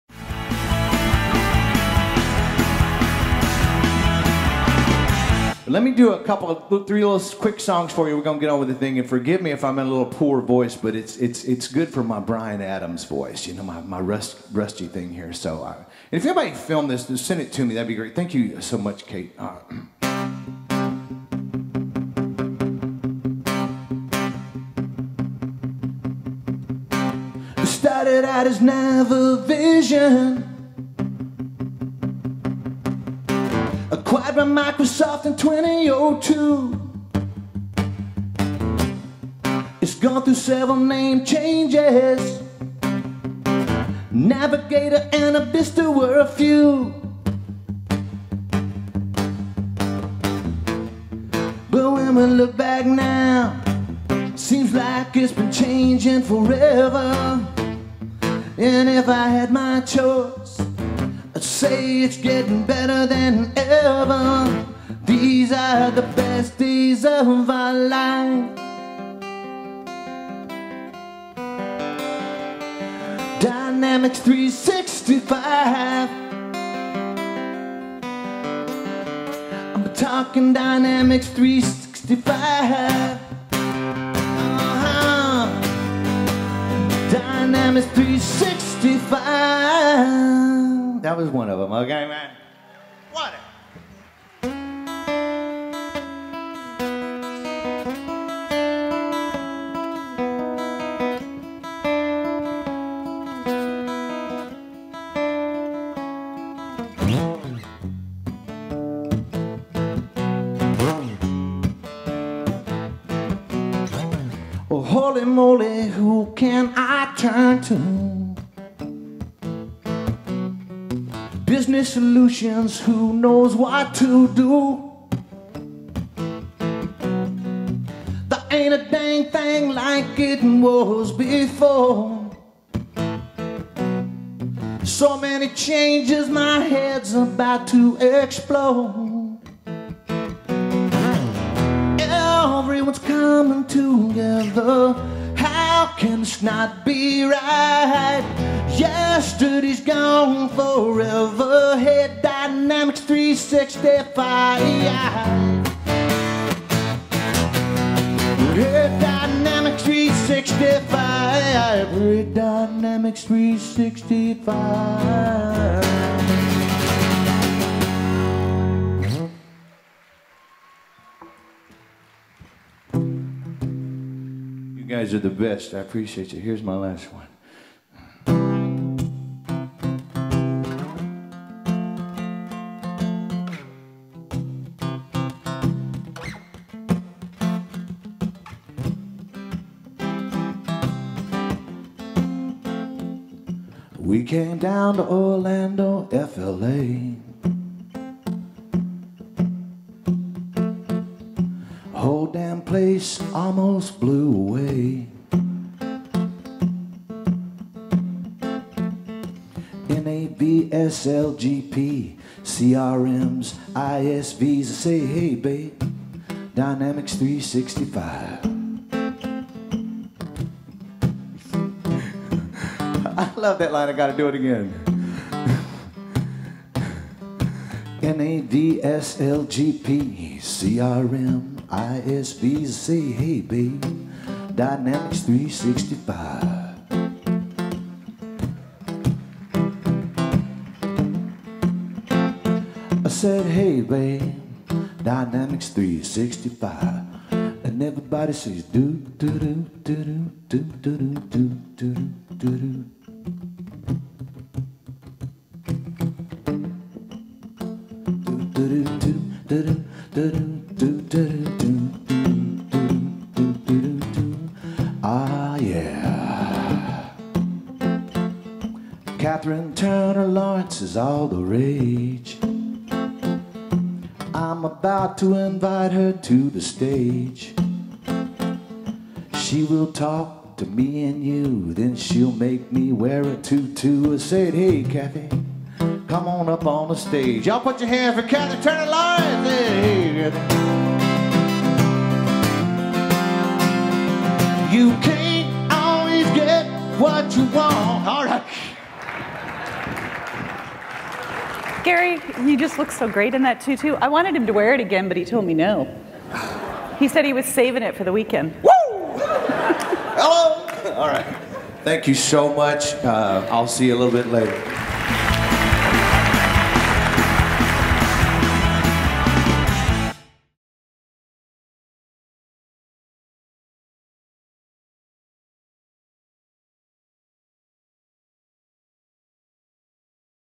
Directions North America 2017 - Songs from Gary Jenkins
Songs from Gary Jenkins at Directions North America 2017 in Orlando, Florida.